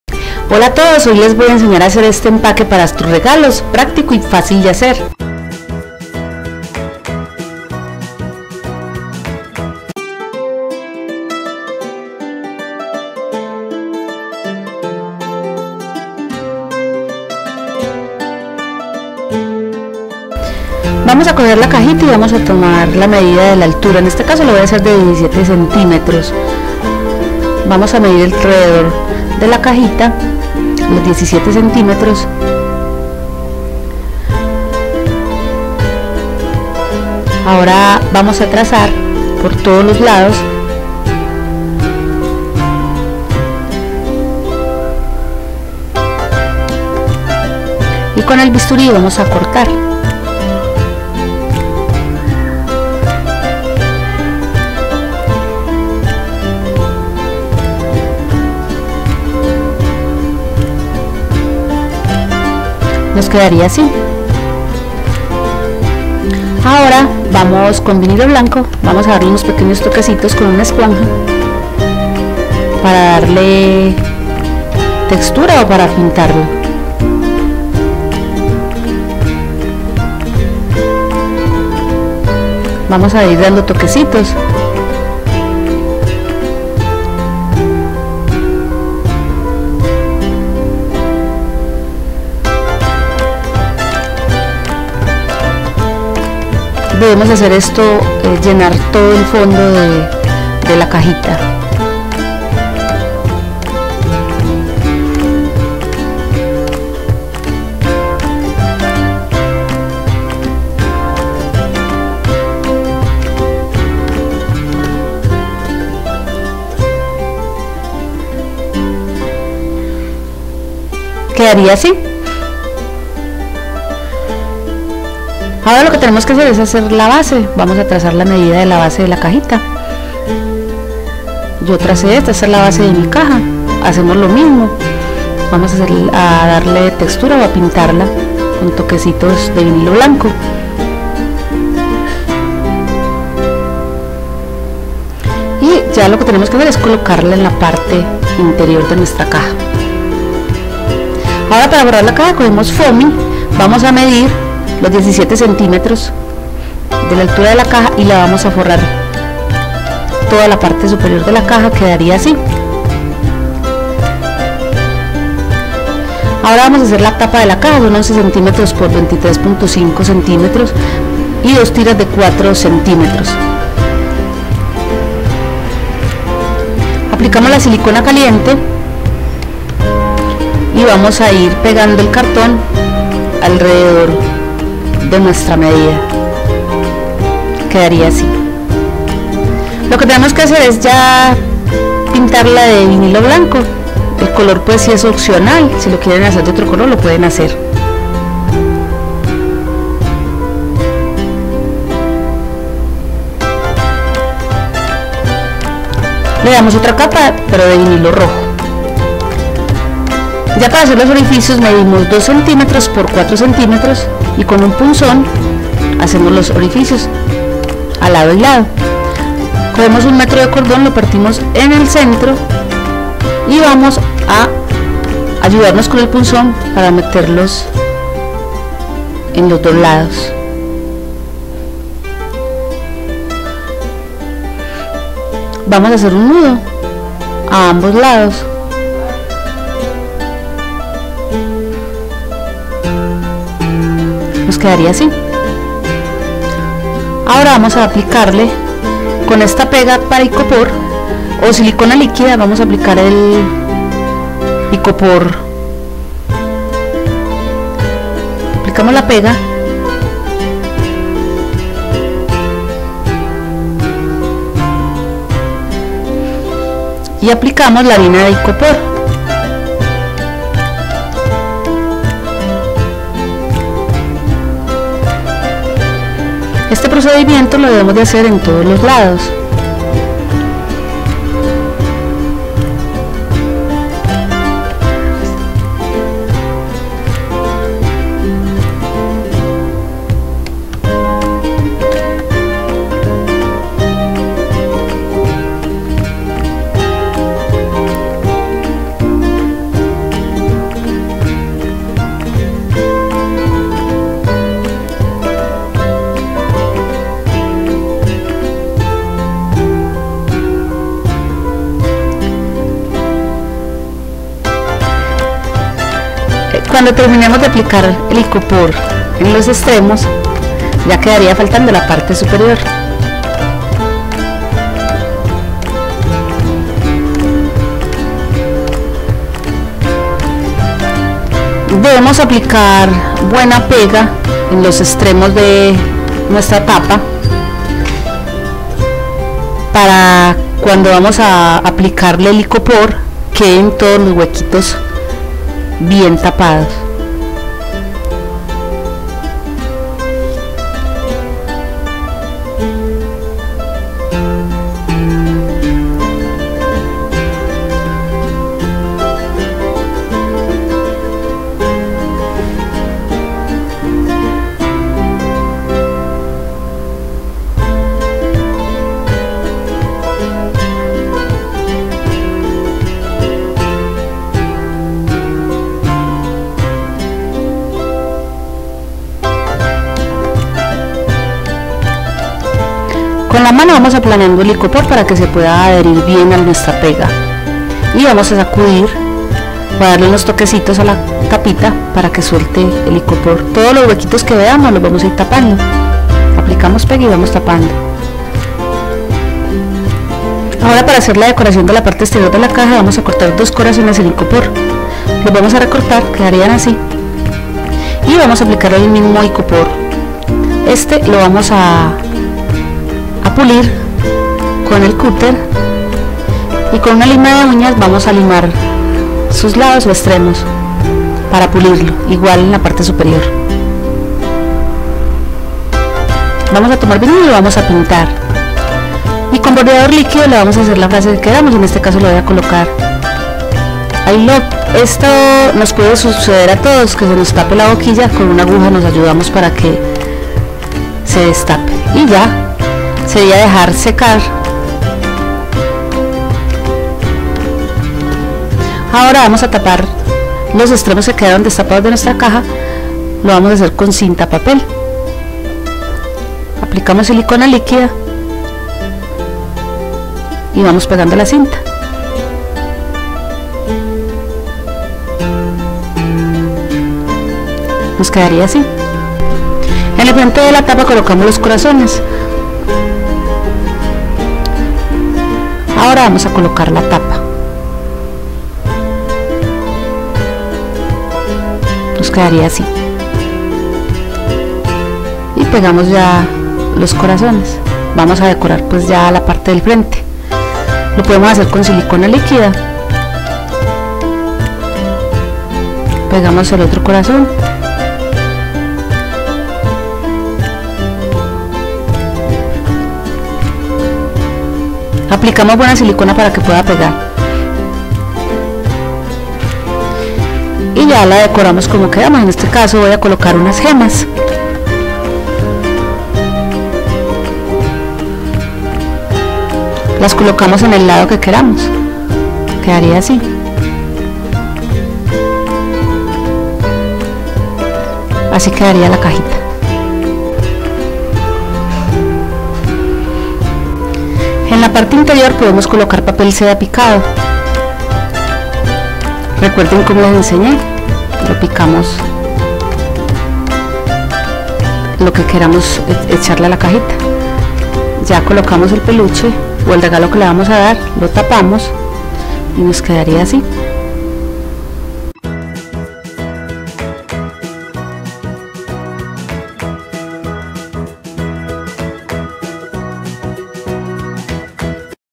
Caja Regalo - CREACIONES BETINA
En este vídeo te explico, como hacer una caja regalo para toda ocasión,la puedes personalizar para toda ocasión.
Esta caja regalo se hace con materiales reciclables y fáciles de conseguir.

DARLE UN like ! si te gusto, dejar un comentario,pero sobre todo compartir este video con tus amigos.
Hasta pronto.